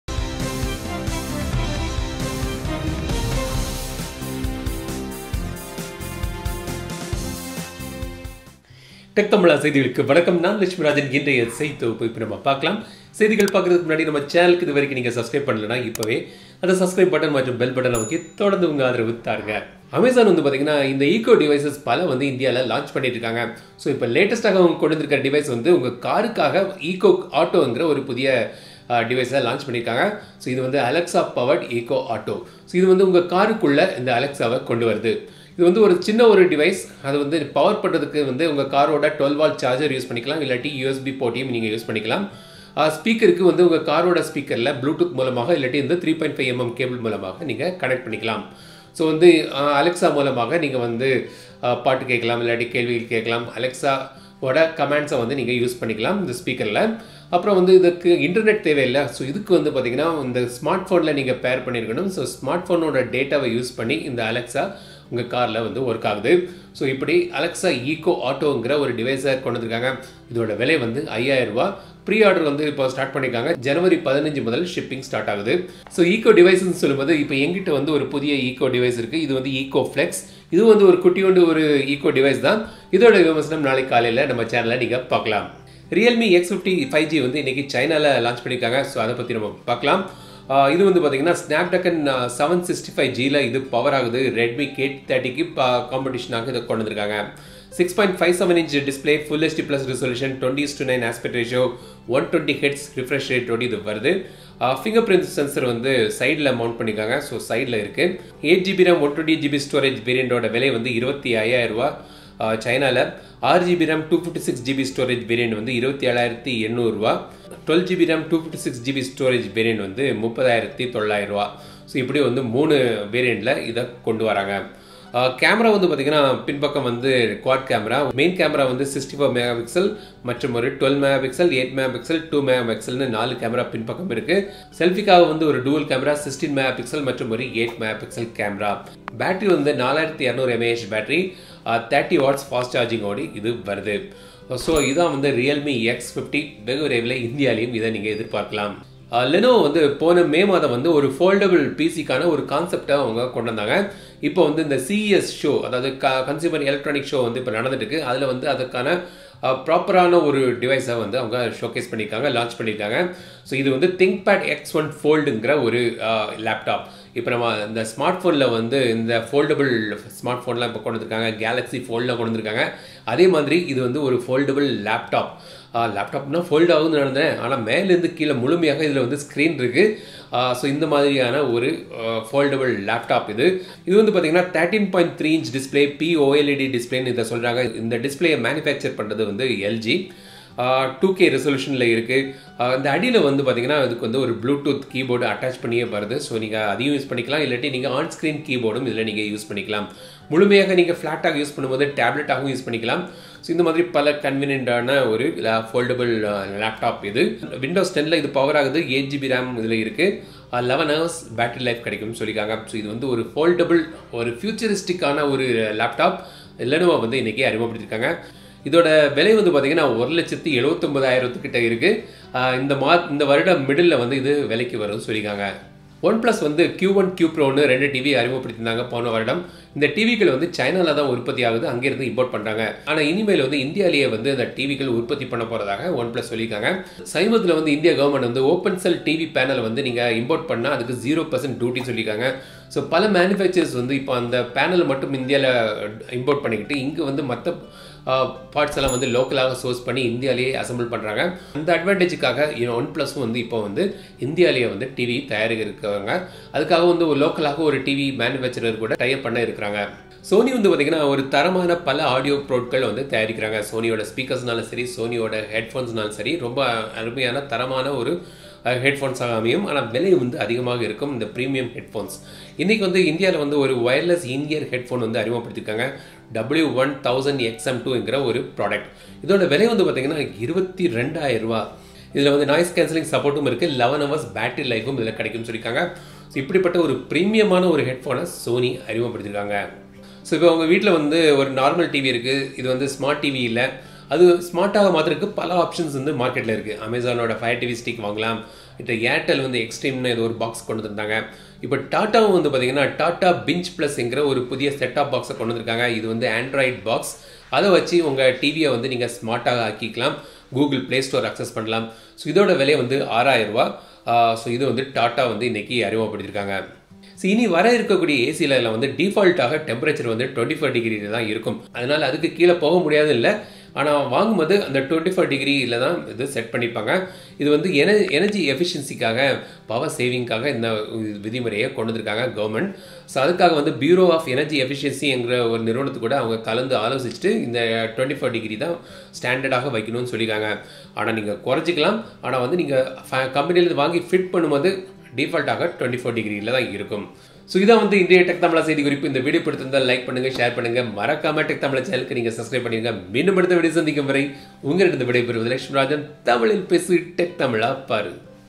கார்-கு Alexa Echo Auto, Tata Binge+ Android Box, Lenovo Foldable Laptop, Realme X50 | டெக் தமிழா
In today's Tech news in Tamil we talk about the newly launched Amazon Echo Auto for Car, Tata Binge plus Android TV box, news from CES 2020, Realme X50 5G launched in China details and more.